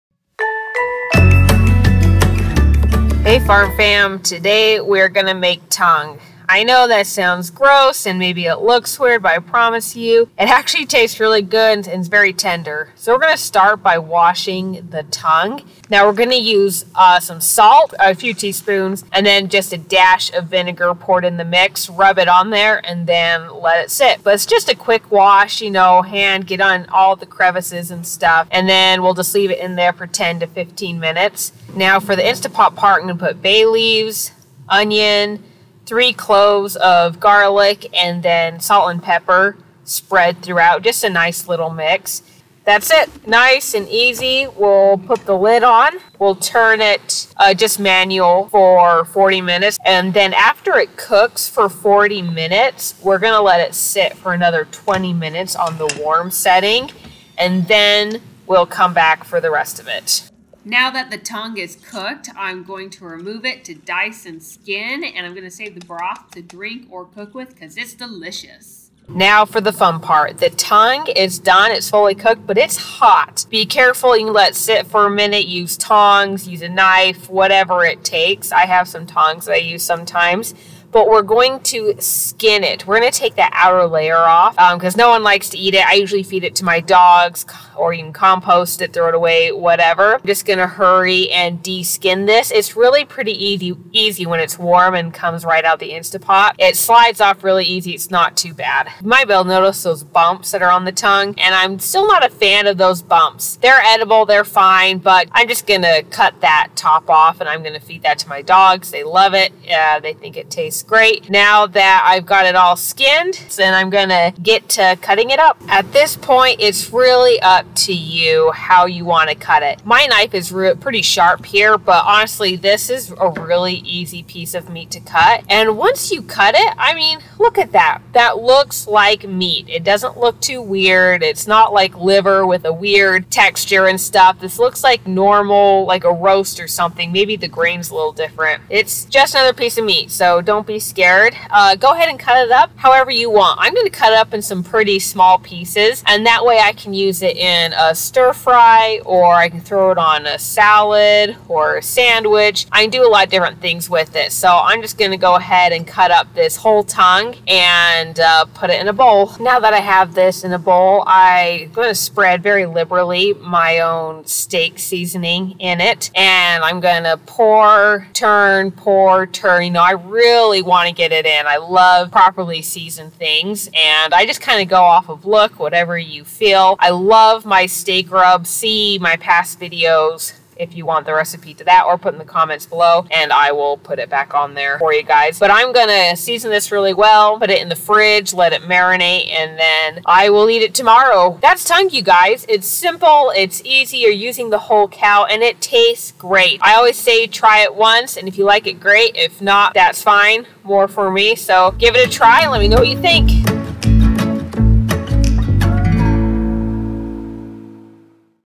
cow tongue
A big thank you to @Viewsontheroad for the amazing recipe. It works like a yummy charm.

This recipe is so easy and simple, I love it.

Clean the tongue:
I put the tongue in a big bowl of water with a splash of vinegar and 2-3 teaspoons of salt. Then I rub the tongue down and do a quick wash in all the nooks and crannies. Then leave it in the mixture for 10-15 minutes.

In the pot ingredients:
1 onion cut in half
3 garlic cloves
2-3 bay leaves

Put the tongue and all the ingredients in the pot and put enough water in to cover the tongue.

Set the instapot to manual and cook for 40 minutes. After that let it sit in the pot for an extra 20 minutes.

After it is cooked pull the tongue out and peal the skin off.

Cut/dice the tongue anyway you want and enjoy!
I like to add my steak rub onto the tongue for extra deliciousness.